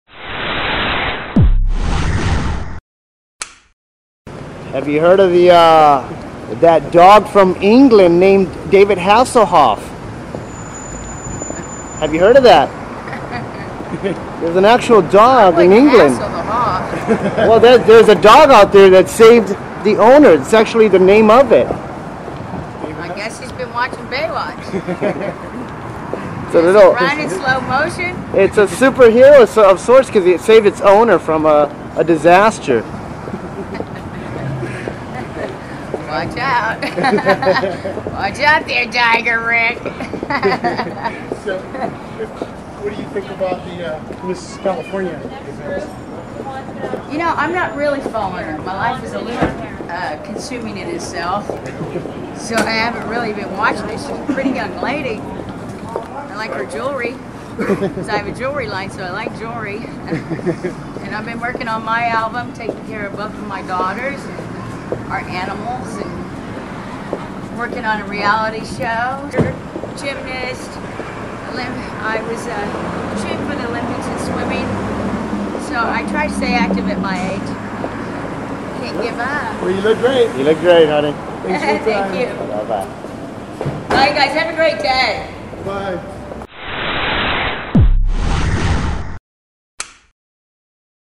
Pamela Bach Walks And Talks On Bedford Drive.
Pamela Bach Talk To Cameras On Bedford Drive in Beverly Hills.